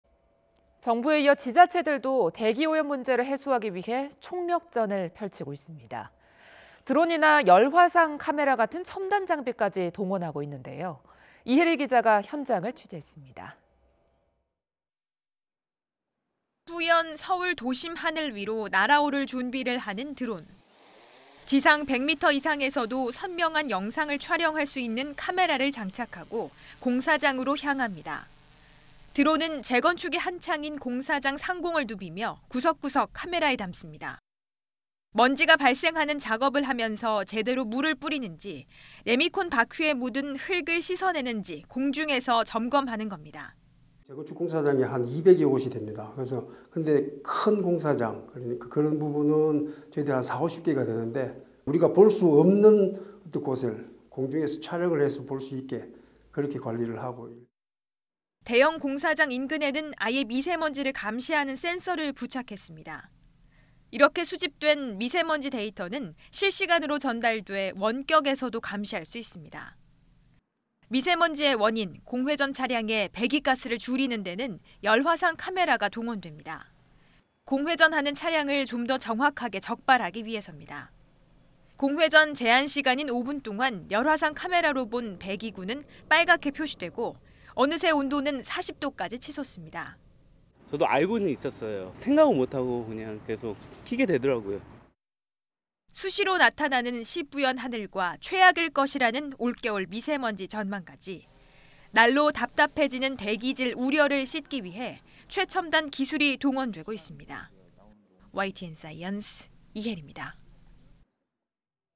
드론·열화상 카메라...'기술'로 뿌연 하늘 씻는다 / YTN 사이언스
[앵커]
청명하다는 가을이지만 국내 오염 물질에 중국발 스모그가 더해지면서 미세먼지와 대기오염에 대한 국민 불안이 커지고 있습니다.

지자체들은 이런 대기오염 문제를 해소하기 위해 첨단 장비를 동원한 미세먼지 줄이기 총력전을 펼치고 있습니다.

이혜리 기자가 현장을 취재했습니다.

[기자]
뿌연 서울 도심 하늘 위로 날아오를 준비를 하는 드론,

지상 100m 이상에서도 선명한 영상을 촬영할 수 있는 카메라를 장착하고 공사장으로 향합니다.

드론은 재건축이 한창인 공사장 상공을 누비며 구석구석 카메라에 담습니다.

먼지가 발생하는 작업을 하면서 제대로 물을 뿌리는지 레미콘 바퀴에 묻은 흙을 씻어내는지 공중에서 점검하는 겁니다.

[최충환 / 서울시 서초구청 푸른환경과장 : (서초구 내) 재건축 공사장이 200여 곳 됩니다. 큰 공사장은 40∼50개가 되는데 우리가 볼 수 없는 곳을 공중에서 촬영해서 볼 수 있게 그렇게 관리를 하고….]

대형 공사장 인근에는 아예 미세먼지를 감지하는 센서를 부착했습니다.

이렇게 수집된 미세먼지 데이터는 실시간으로 전달돼 원격에서도 감시할 수 있습니다.

미세먼지의 원인, 공회전 차량의 배기가스를 줄이는 데는 열화상 카메라가 동원됩니다.

공회전하는 차량을 좀 더 정확하게 적발하기 위해섭니다.

공회전 제한시간인 5분 동안 열화상 카메라로 본 배기구는 빨갛게 표시되고 어느새 온도는 40도까지 치솟습니다.

[공회전 단속 적발 시민 : (공회전 단속하는 건) 알고는 있었어요…. 생각을 못 하고 계속 (시동을) 켜게 되더라고요.]

수시로 나타나는 시뿌연 하늘과 최악일 것이라는 올겨울 미세먼지 전망까지,

날로 답답해지는 대기 질 우려를 씻기 위해 최첨단 기술이 동원되고 있습니다.